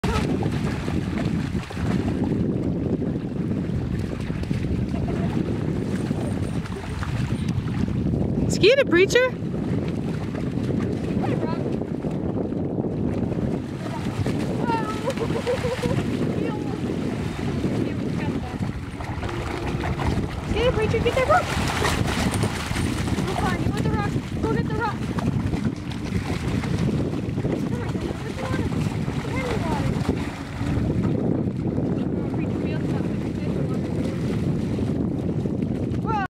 Cane Corso's playing in water
Preacher, Cashmere and Eclipse playing in the lake. We like to hike with our dogs for long periods of time to condition them and test their health and stamina.

01198708abc987